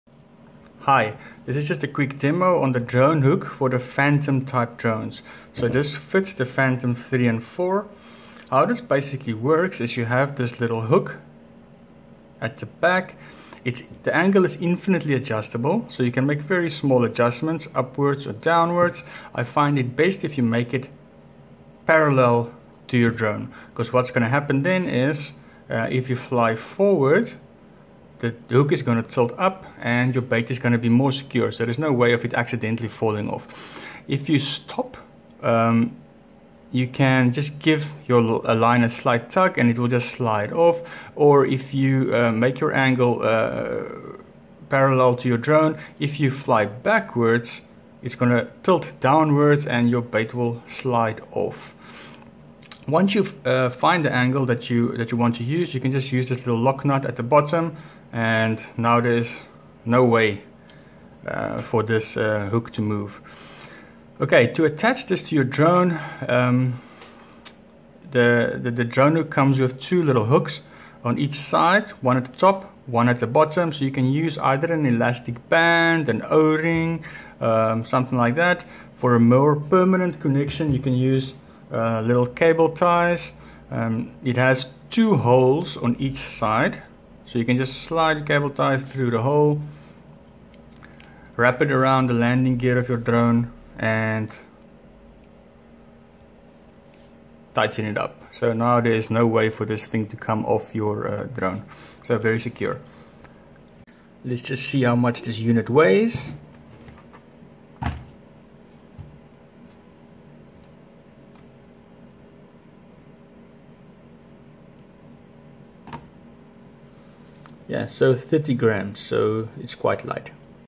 Dronehook for Phantom type drones
A short demo on how to use the Dronehook for DJI Phantom 3 and 4 drones.